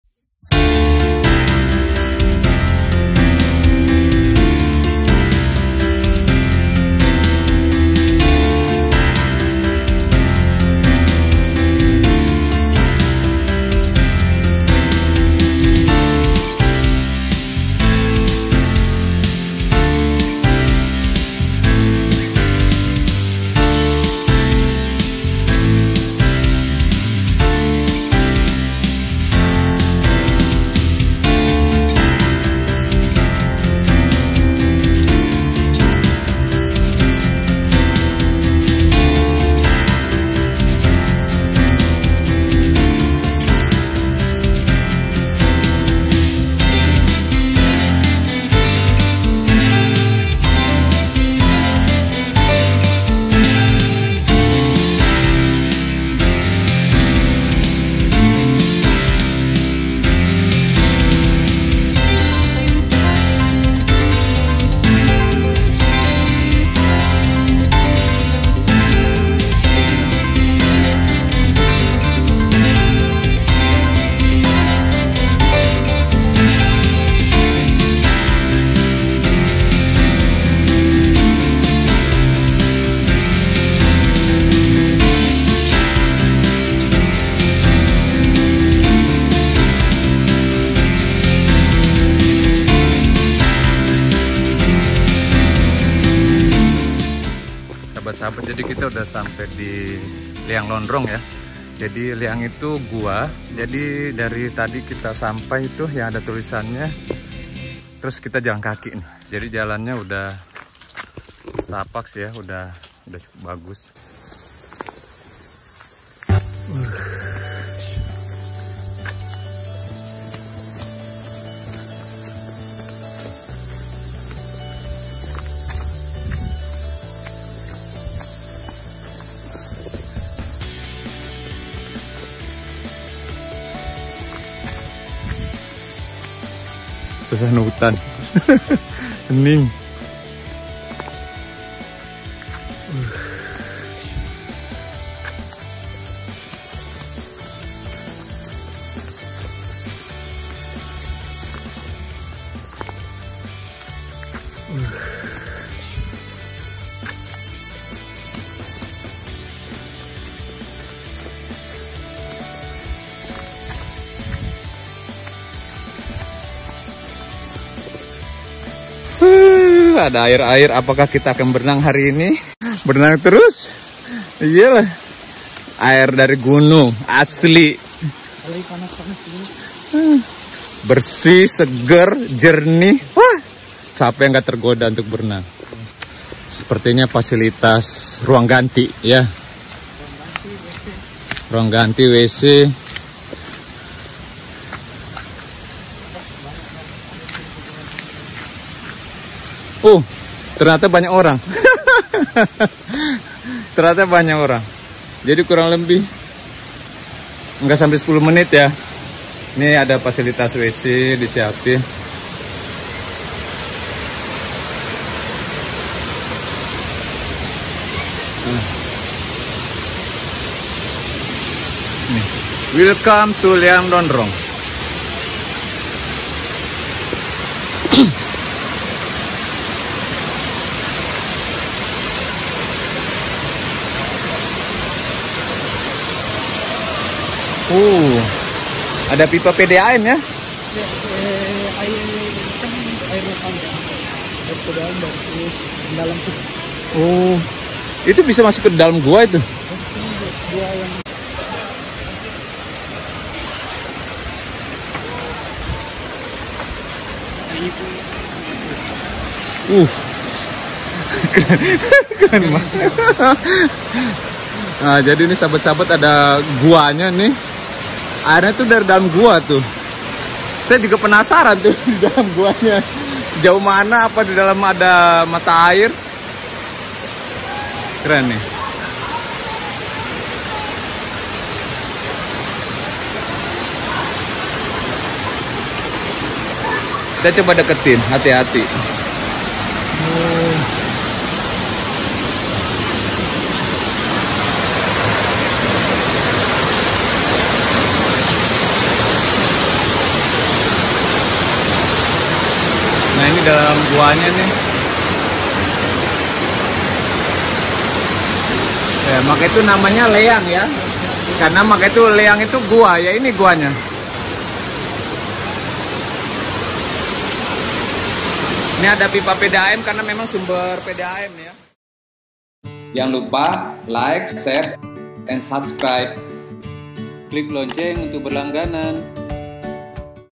GEOSITE LEANG LONDRONG BALOCCI KAB. PANGKAJENE DAN KEPULAUAN Geopark Maros Pangkep
GEOSITE LEANG LONDRONG BALOCCI Kabupaten PANGKAJENE DAN KEPULAUAN Geopark Maros Pangkep Provinsi Sulawesi Selatan
Gua dengan sistem sungai bawah tanah sepanjang 7 km, terhubung ke sistem hidrologi batuan vulkanik Gunung Bulu Saraung disebelah utara sebagai sebuah penampungan. Debit air di dalam gua stabil sepanjang tahun menyebabkan dinding gua menjadii lebih halus dan tidak memiliki speleothem, sebagian air keluar pada kaki bukit karst (sungai bawah tanah). Bentuk dinding yang halus memungkinkan untuk menelusuri gua dengan kano.
Untuk masuk ke destinasi wisata ini dikenakan retribusi Rp. 5.000/orang.